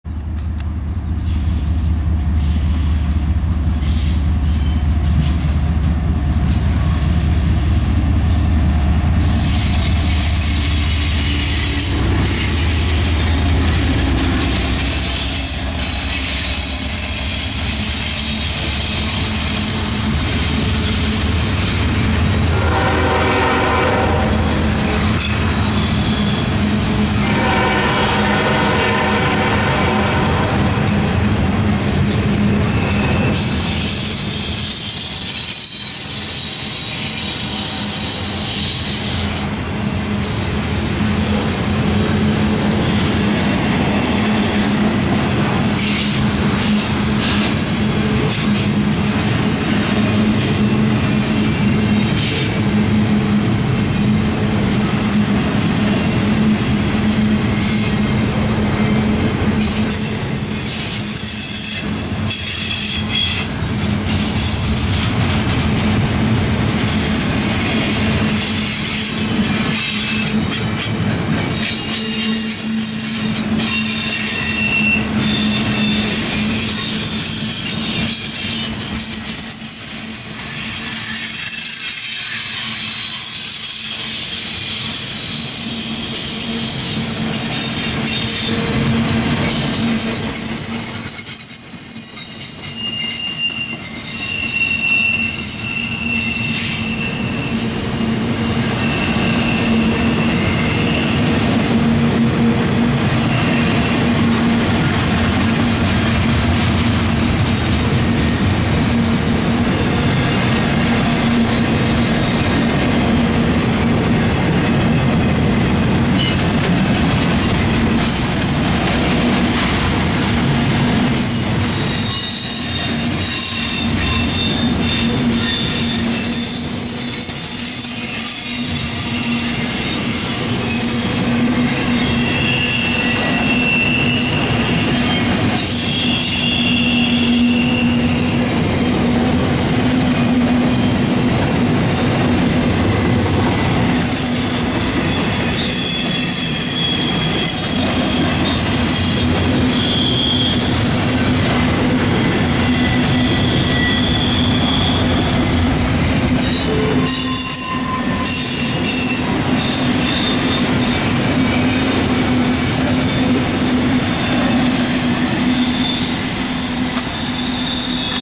Another Norfolk Southern stack train through Alliance Ohio
Nothing special on this stack train also, NS 7666 an ES44DC and 2722 a SD70M-2 were the power for the train.